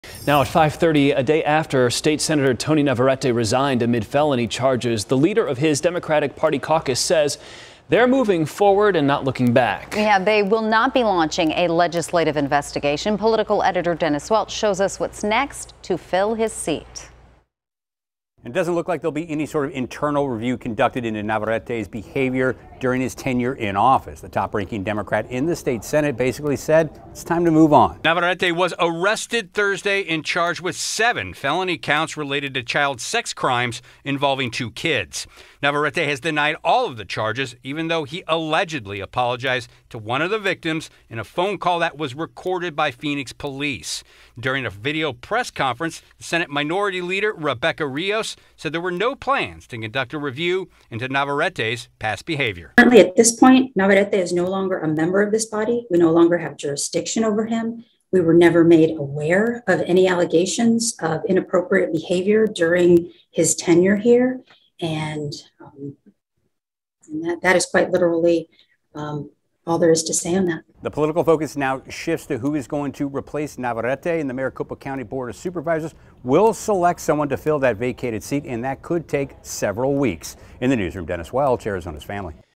State Democrats look to move forward after Navarrete arrest
State Senate Democrats say it's time to move on after Tony Navarrete was arrested on molestation charges and the party is working to fill his seat in District 30.